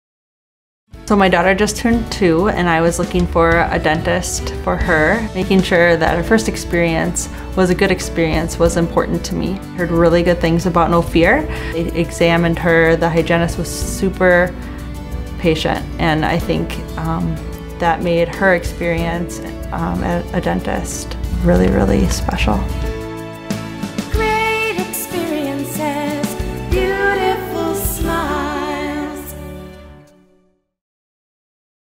Daughter's First Dentist Appointment at Madison No Fear Dentistry
We specialize in providing great experiences to patients of all ages.  Hear Alicia share her personal story on finding the right Madison, WI dentist for her daughter.